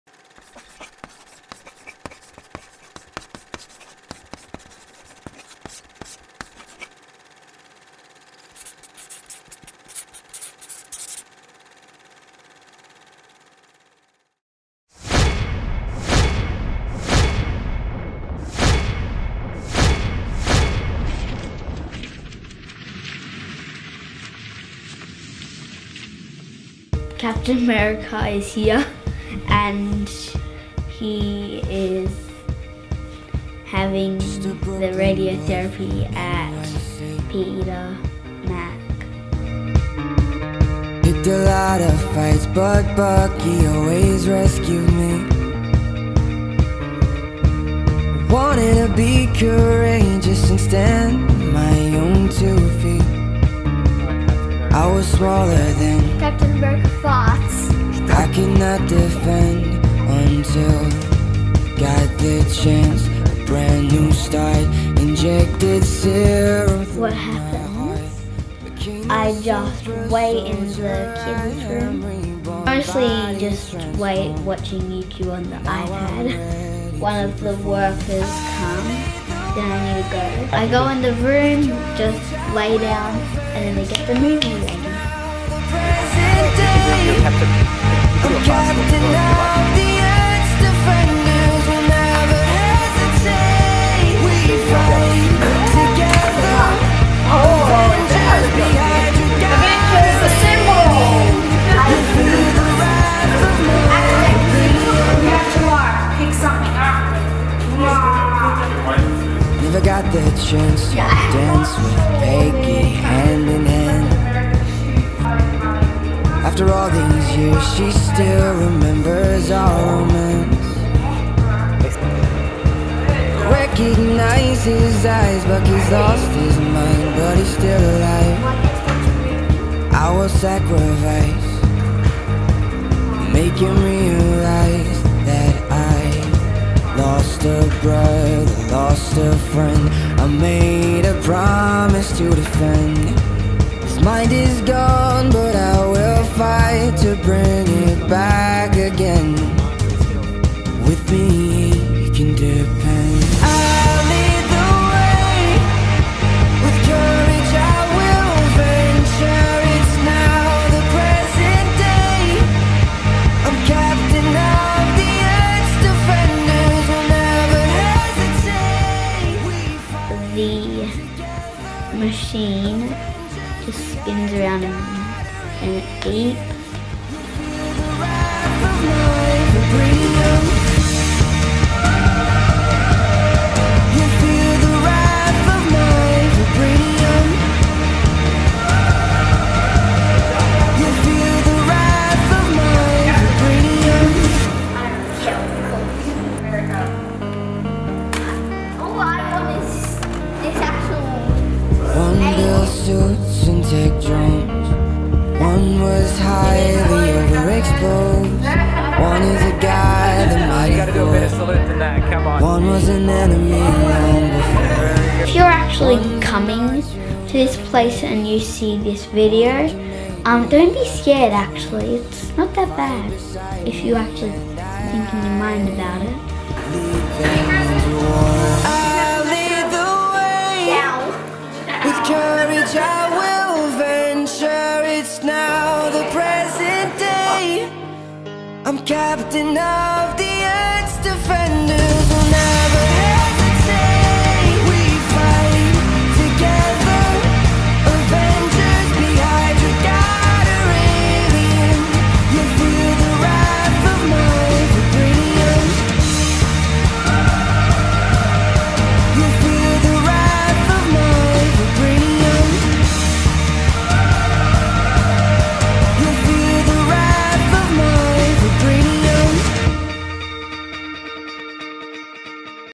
Marcus' Movie
Watch Marcus' radiation therapy journey at Peter Mac